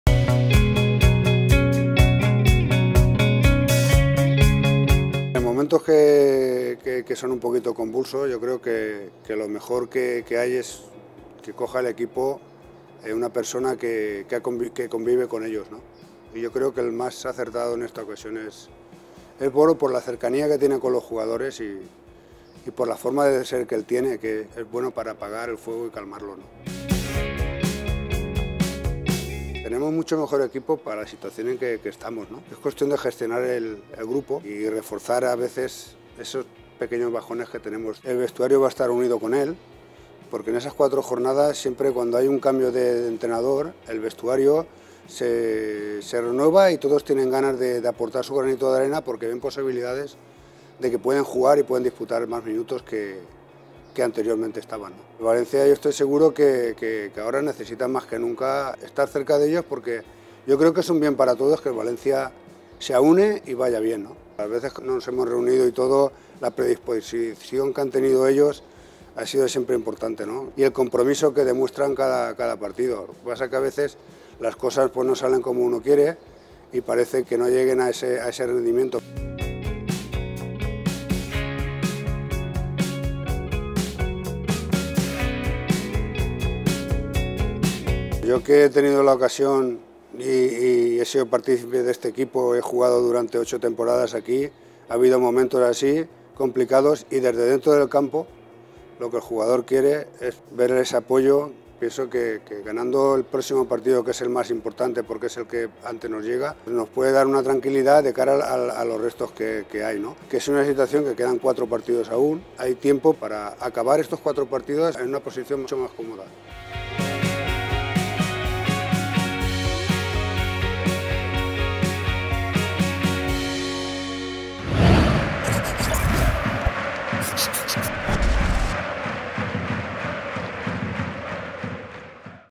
MIGUEL TENDILLO MUESTRA SU CONFIANZA HACIA VORO Y LA PLANTILLA
Miguel Tendillo, embajador del Valencia CF, ha analizado en VCF MEDIA la situación del equipo, que este domingo a las órdenes de Voro buscará reencontrarse con la victoria en el compromiso ante el Real Valladolid: “He vivido situaciones como esta, el jugador lo que necesita es ver apoyo”.

The Valencia CF ambassador believes in Voro and the squad.